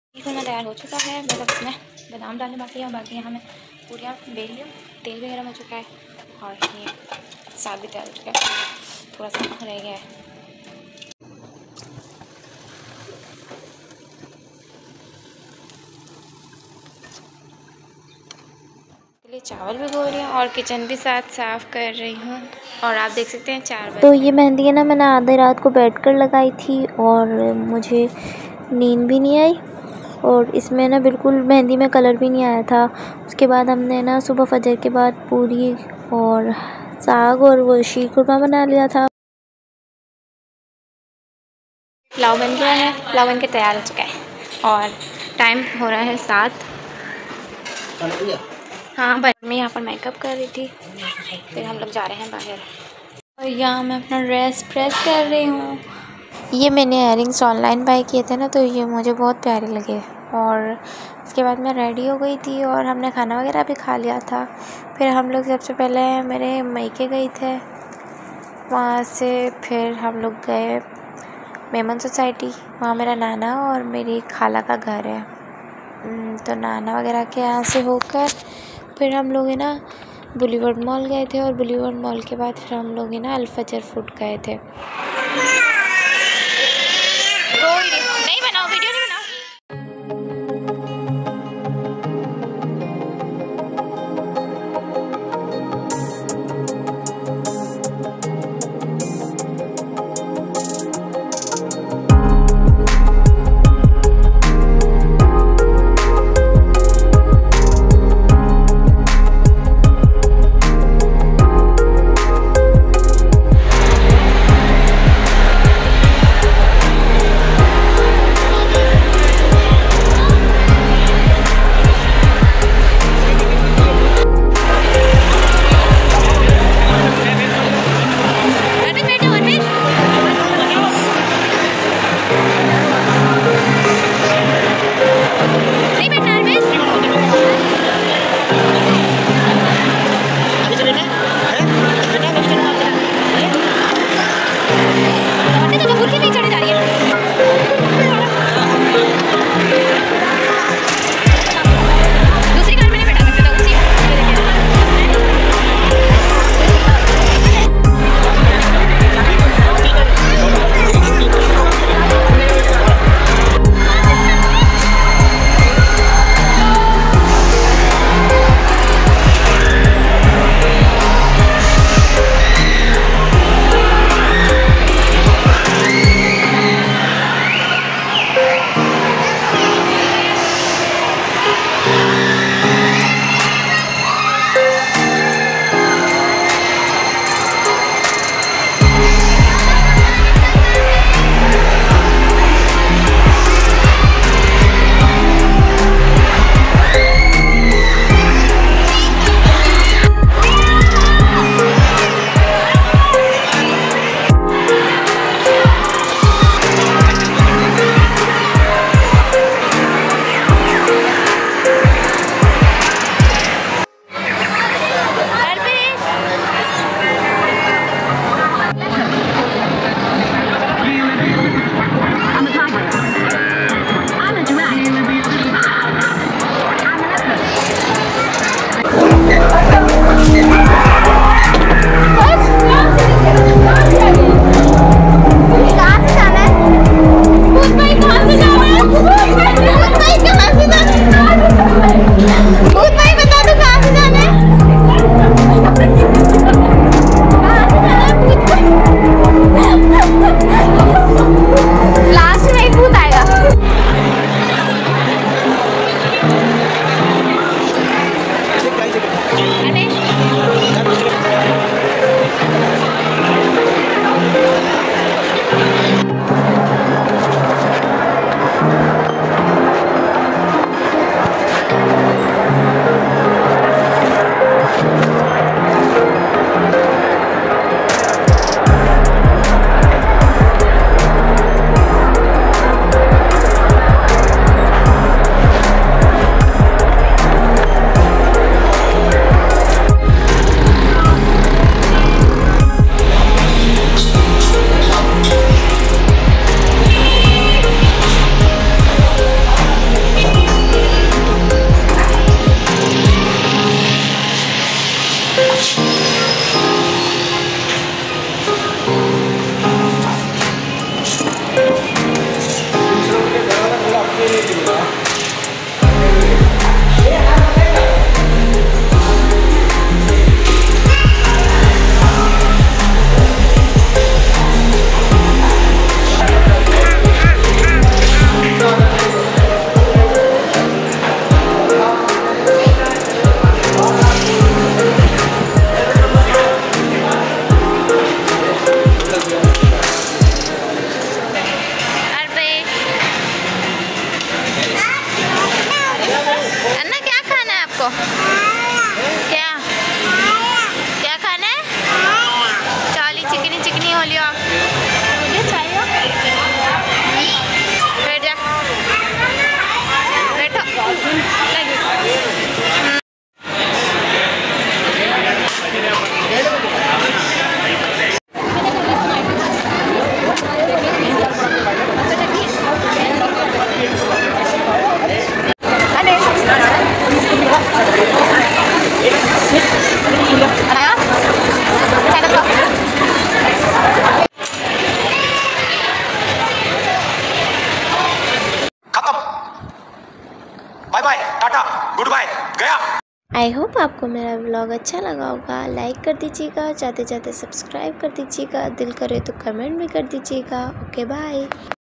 eid Mubarak 2022 || sag pori bnai || boulevard mall gaye || alfajr gaye❤eidmubarak2022
me ne bhoot se kia maze ka sawal

is se phone bhi me yahn gai thi tb to bhut hi funny seen how tha😆